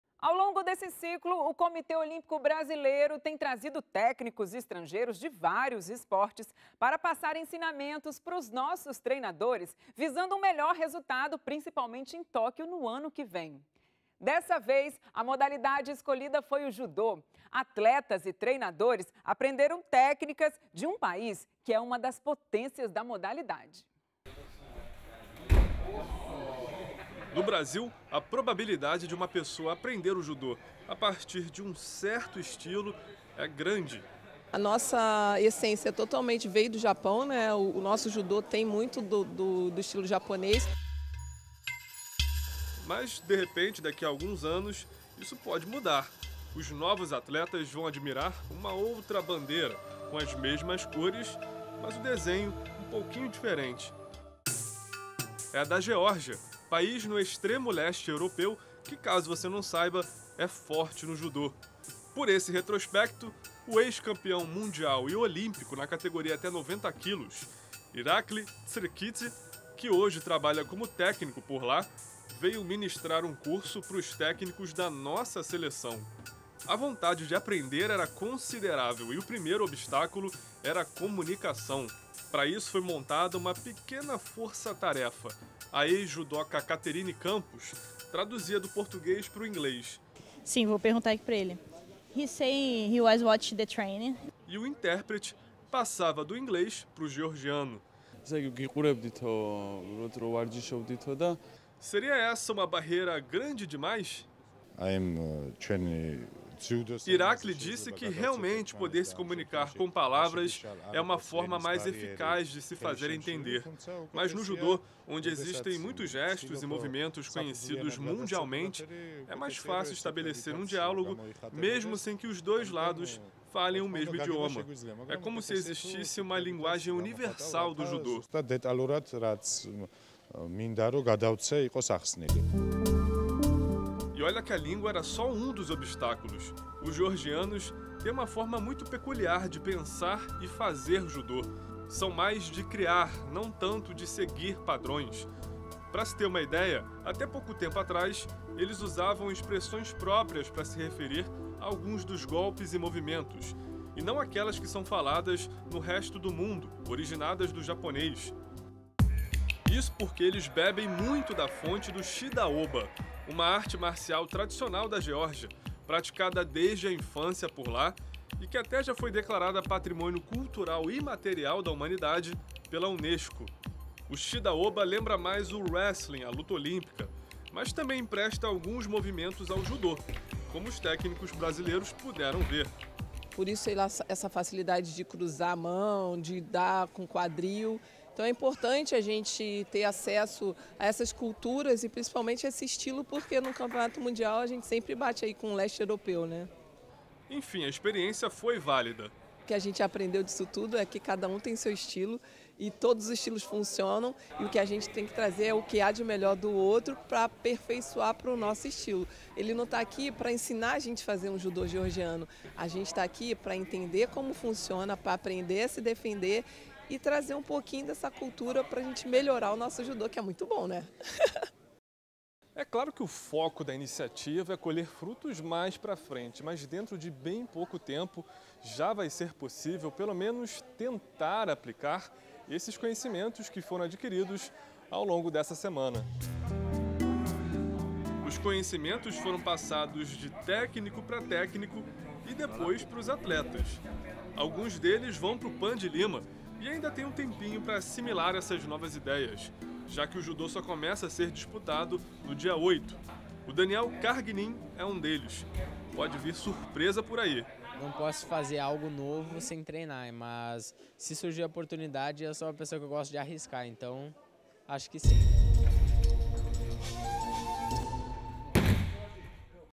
COB traz técnicos estrangeiros para ensinar judocas
Ao longo desse ciclo, o Comitê Olímpico Brasileiro tem trazido técnicos estrangeiros de vários esportes pra passar ensinamentos pros nossos treinadores, visando um resultado melhor principalmente em Tóquio, no ano que vem. Dessa vez a modalidade escolhida foi o judô.  Atletas e treinadores aprenderam técnicas de um país que é uma das potências da modalidade.